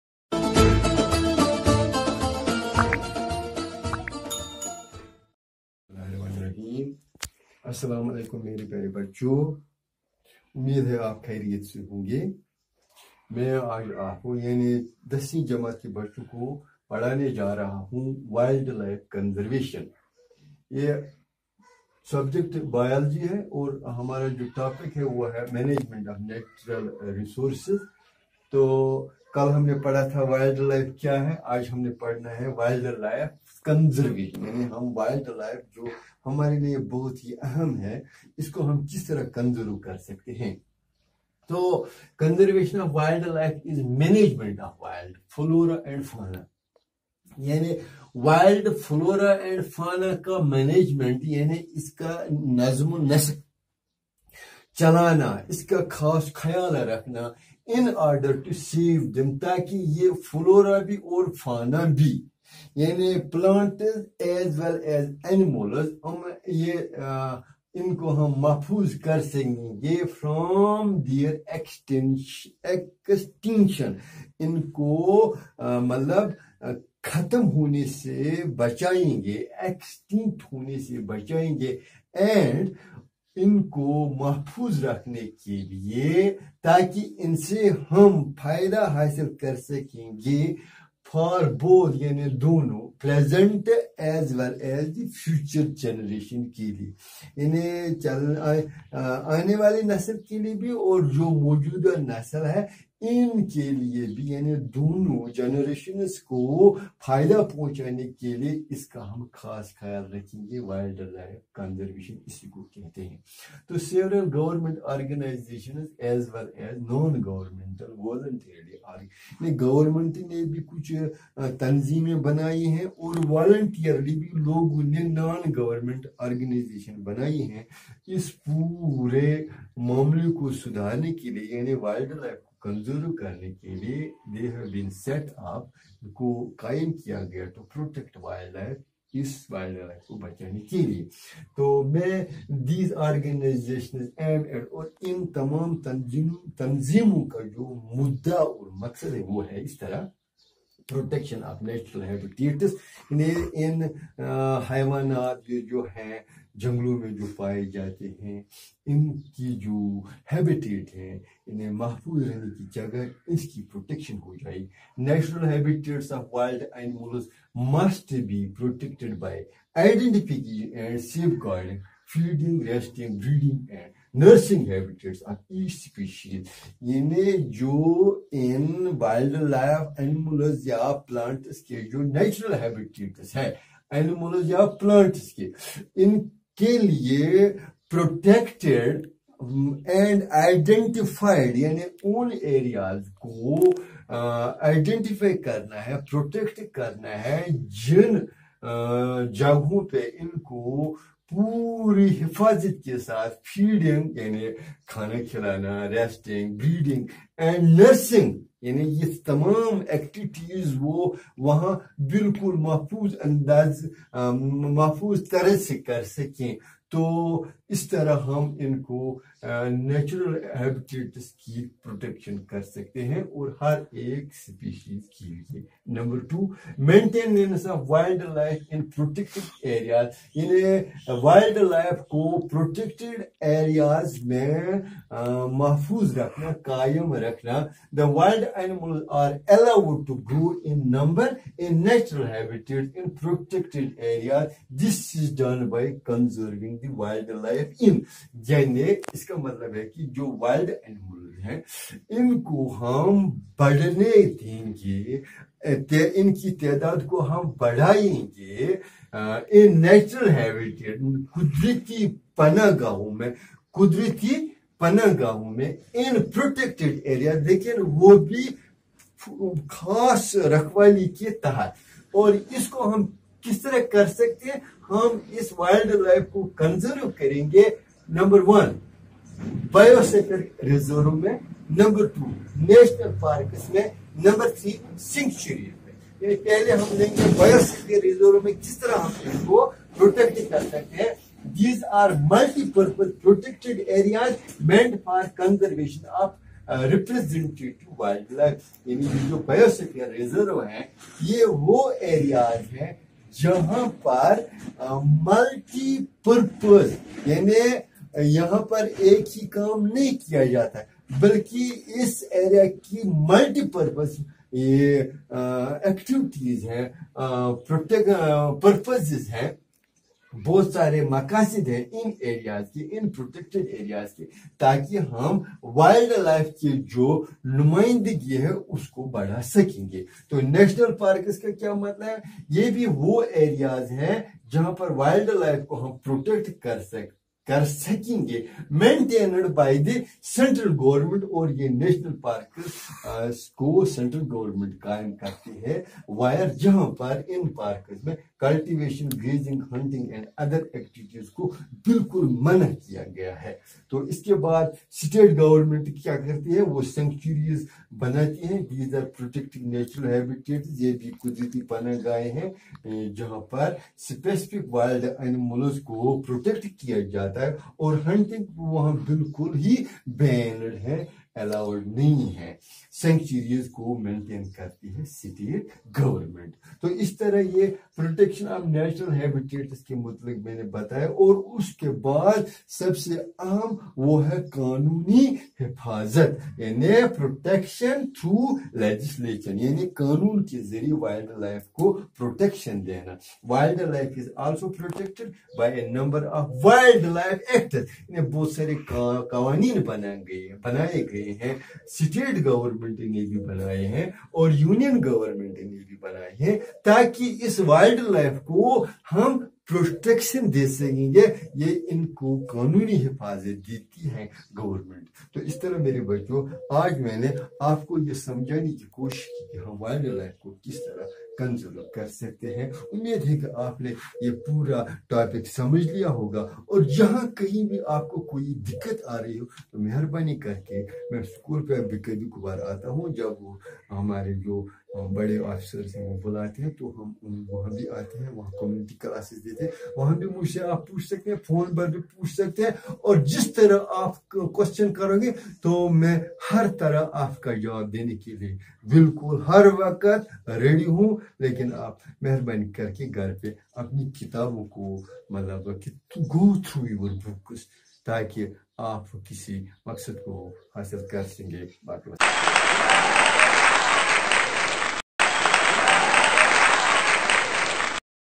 Class: 10th. Sub: Biology. Topic: Conservation of Forests (Management of Natural Resources)
By: Gh. Mohi Ud din Sheikh (Master BHSS HARIL)